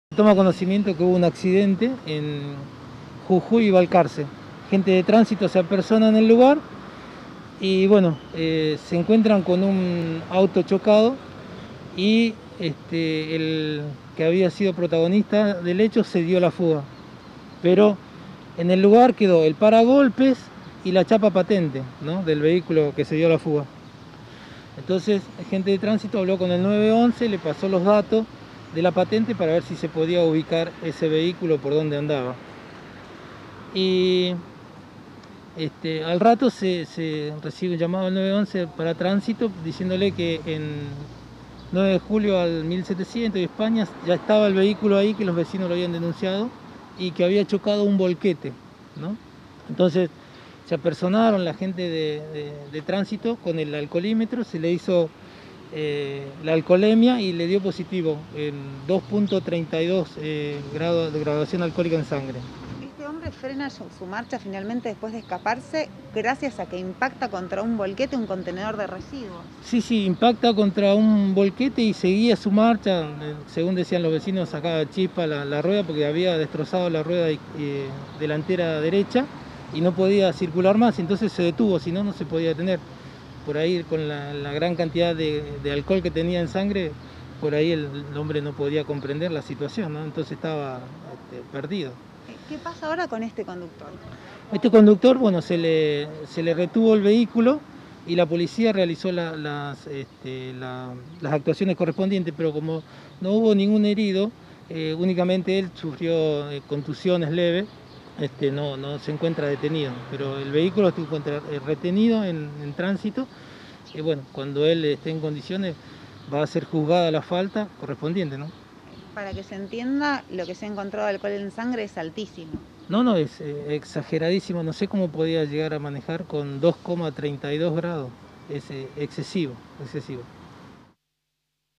Alcoholizado fugó, chocó y lo detuvieron. El test arrojó 2.32 grados de alcohol en sangre.
EL TEST ARROJÓ 2,32 GRAMOS DE ALCOHOL EN SANGRE
RODOLFO ACOSTA GOROSITO – SUB SEC. CONTROL Y CONVIVENCIA

Conductor alcoholizado se fugó, chocó y quedó detenido en el centro
El operativo se inició en Pichincha donde el sujeto chocó un vehículo. Fue detenido por la Policía en 9 de Julio al 1700. El test de alcoholemia le dio 2,32 gramos de alcohol por litro de sangre.
El hombre chocó dos veces en el centro antes de que pudieran detenerlo, el test de alcohol en sangre le dio muy elevado. Muy por encima de lo que permite la normativa incluso antes de que tuviera vigencia la de alcohol 0.